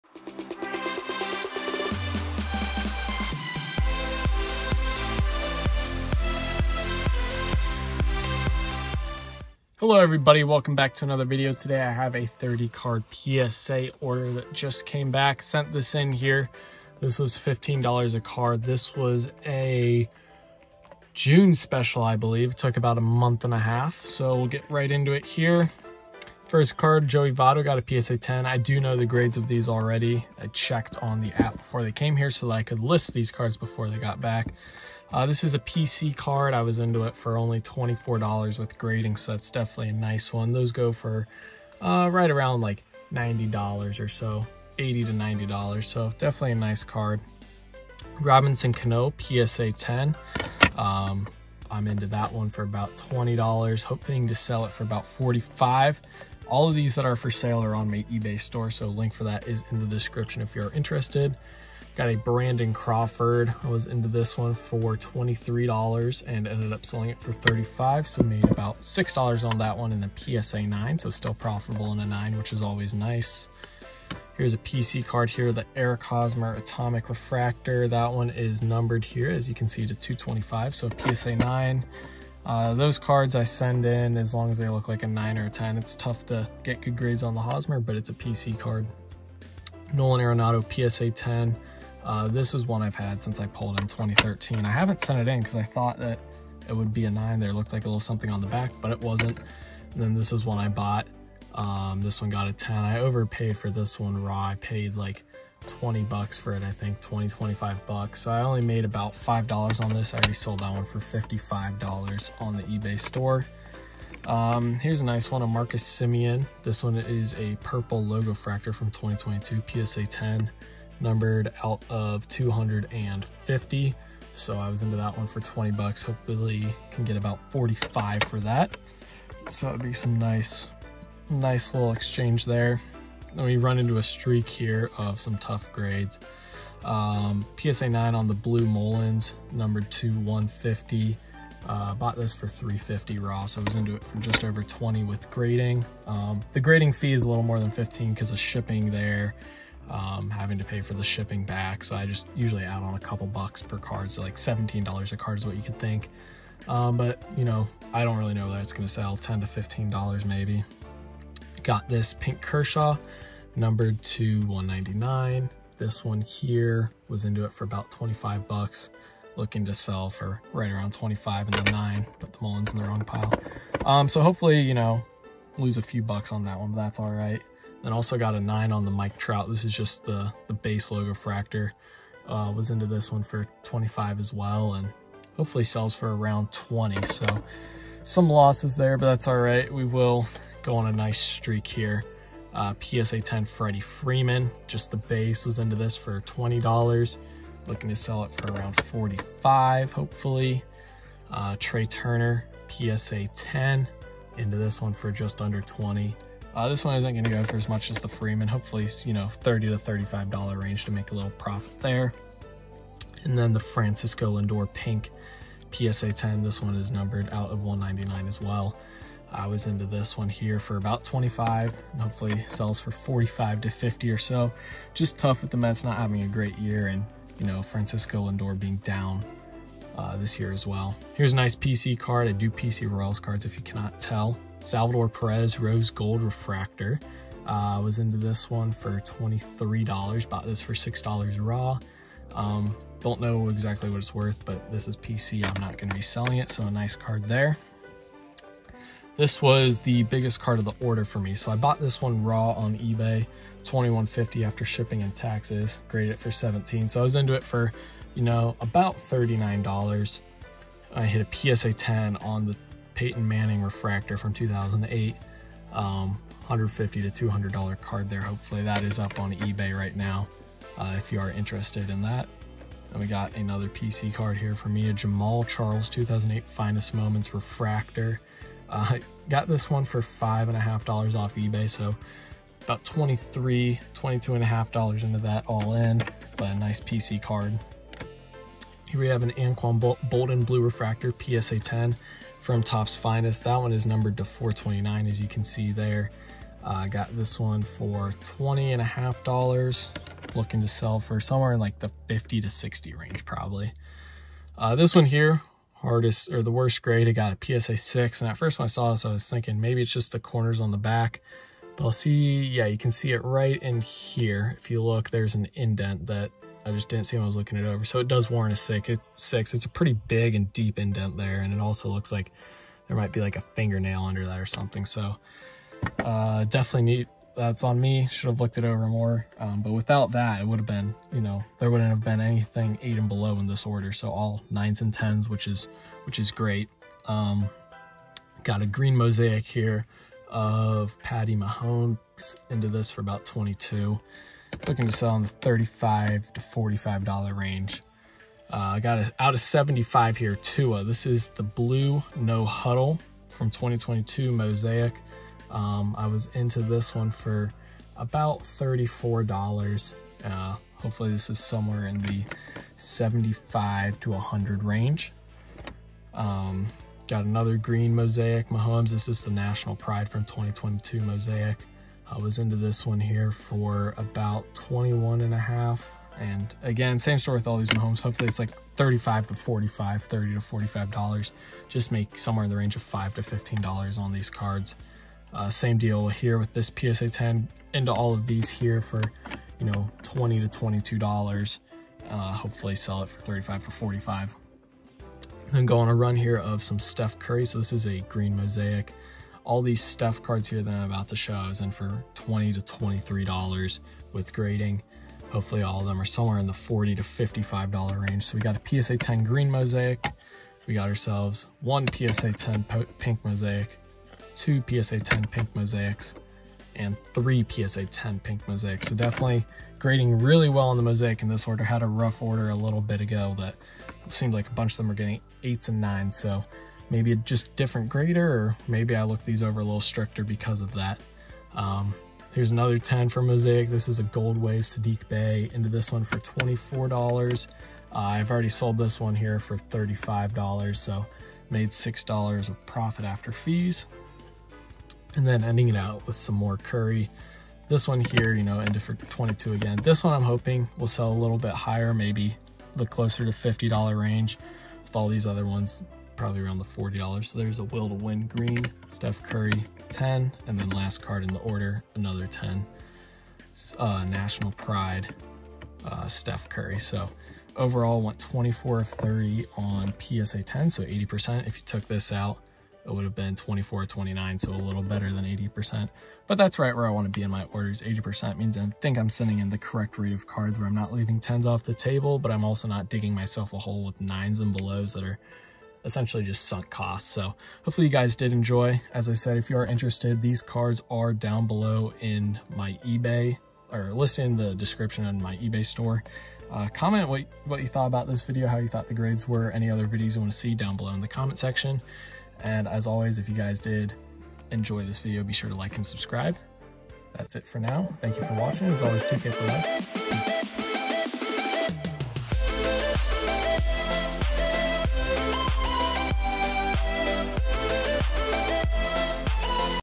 My 30 Card PSA Submission Came Back!
I got my 30 card PSA submission back and did very well. What do you think of the cards?? Many of these cards will be in my eBay store which is linked down below. Comment your favorite card down below. I hope you enjoyed!

Be sure to check out my eBay store down below  ---v

eBay Store